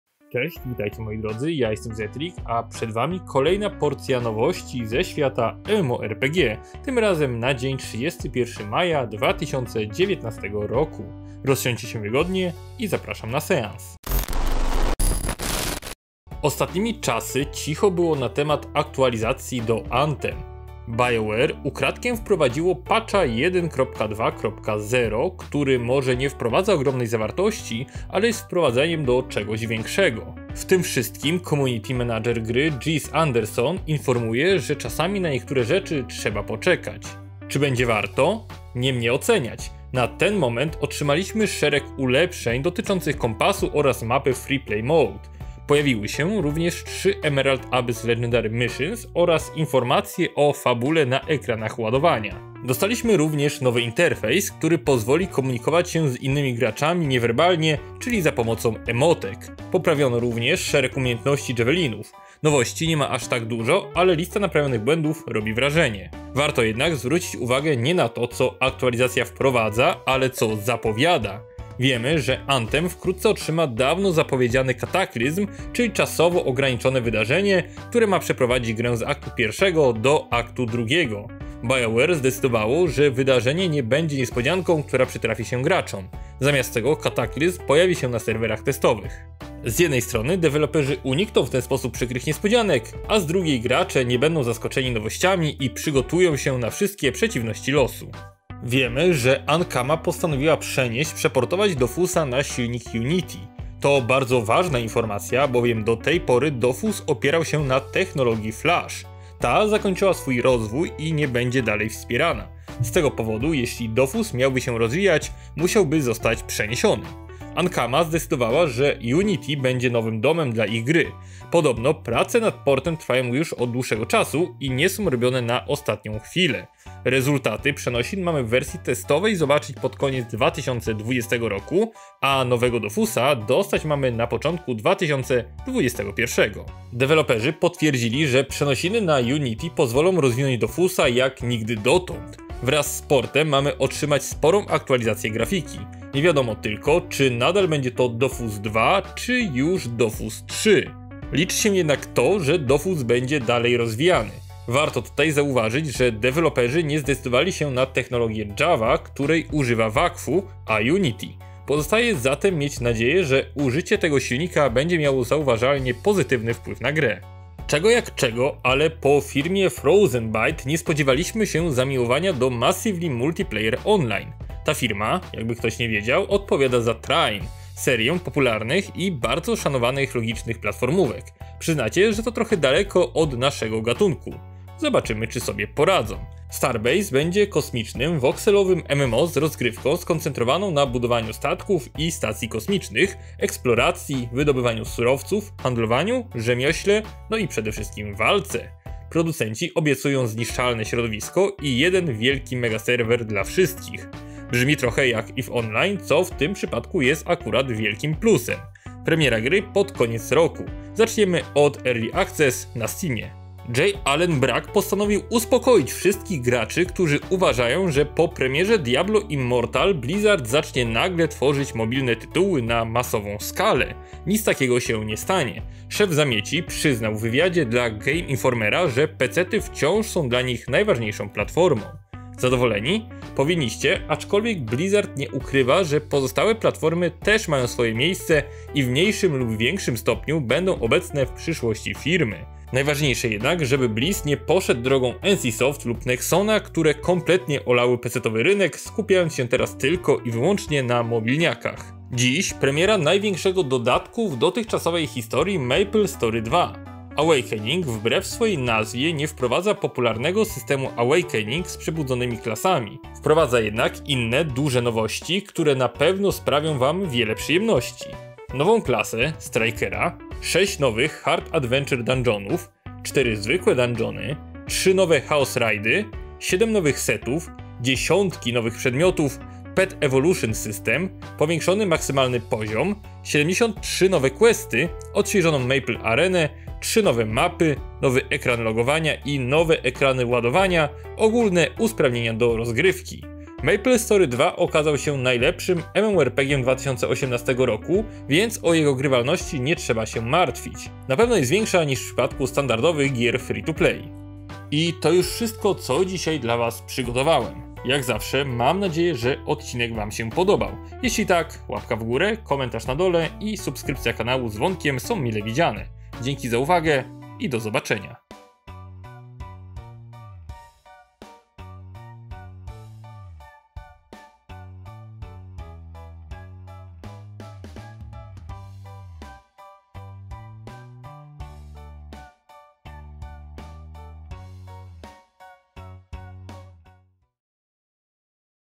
MAPLE STORY 2 Z OGROMNĄ AKTUALIZACJĄ oraz... - MMONews 31.05.2019
Maple Story 2 to jeden z najlepszych MMORPGów 2018 roku. Dziś doczekał się ogromnej aktualizacji. Co na was czeka? Przekonajcie się, oglądając te daily newsy!

ZAPRASZAMY DO OGLĄDANIA INNYCH FILMÓW

KUP FOTEL RAZEM Z NAMI!